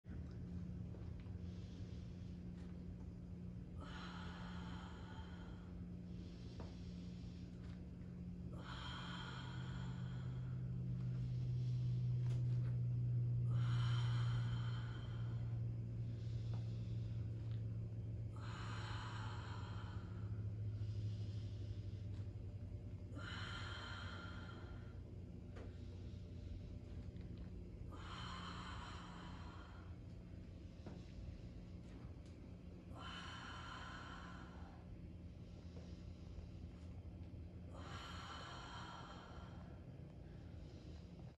Alternating Bird Dog Exercise
Form check: Maintain a neutral spine as you extend opposite side limbs. Exhale as you extend. Avoid shifting or leaning. Brace your core and maintain stability throughout the set.